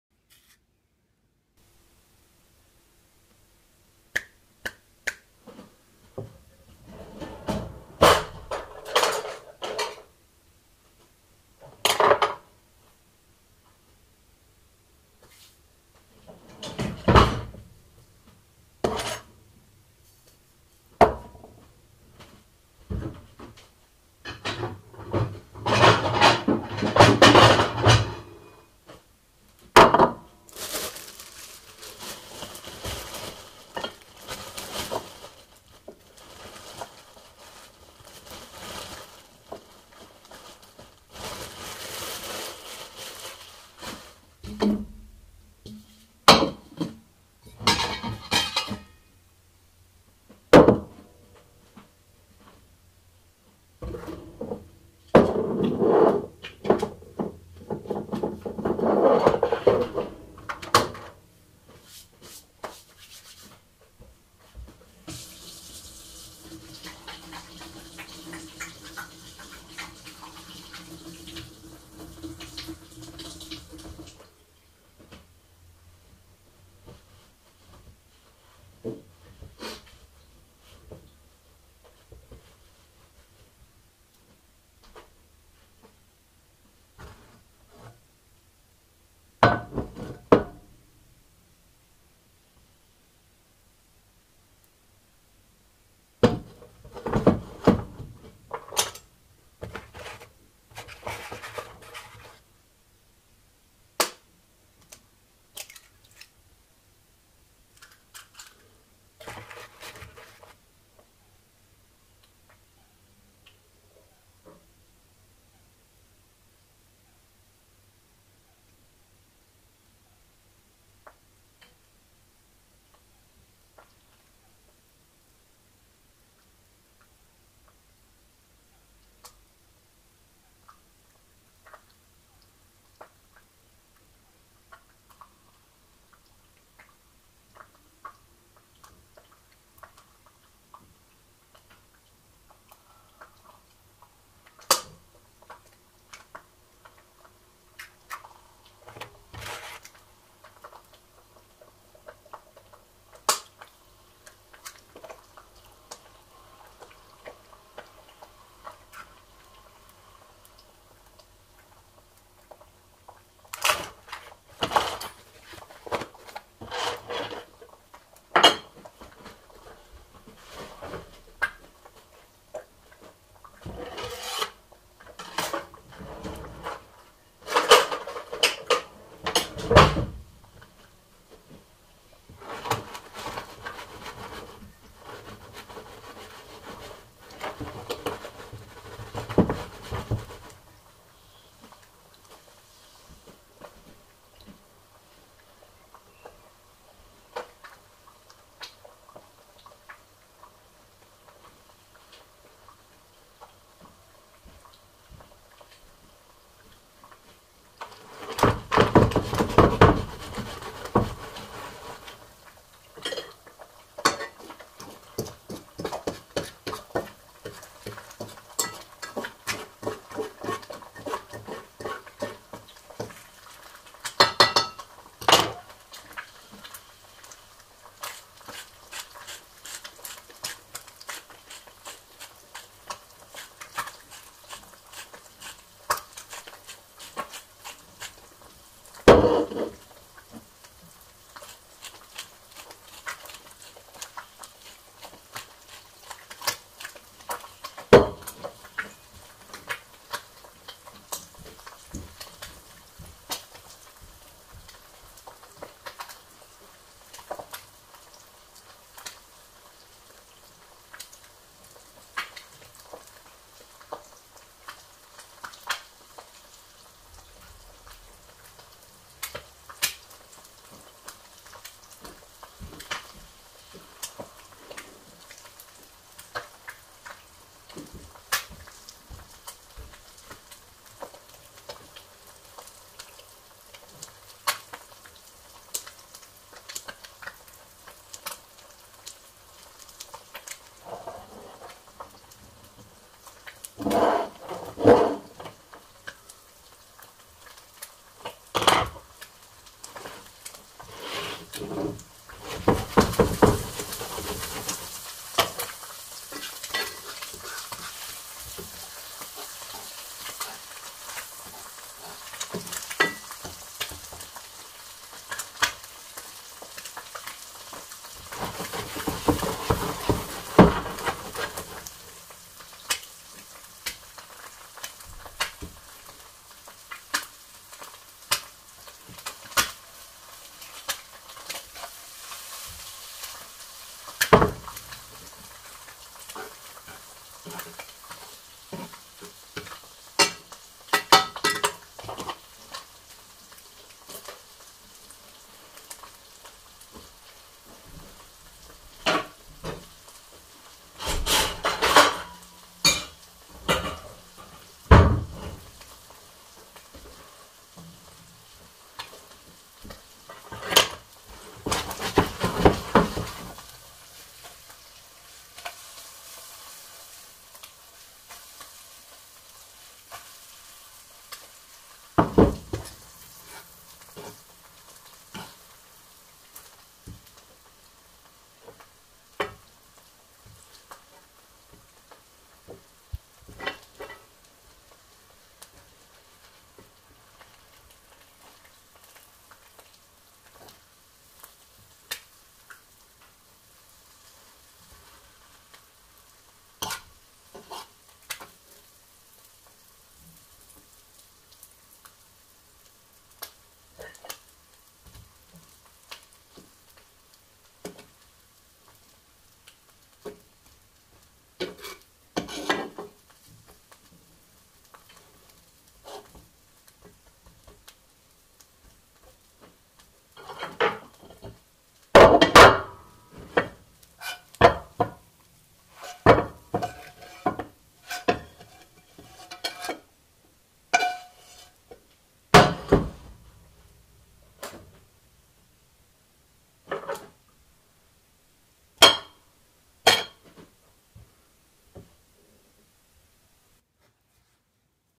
ASMR soundscape: Small spaces: How do you want your eggs?
Listen with headphones, no whispering sounds in this video. I think there is something about being concentrated on what you are doing, In this video i am concentrated on baking you eggs.
You can close your eyes if you want to. Might be relaxing :)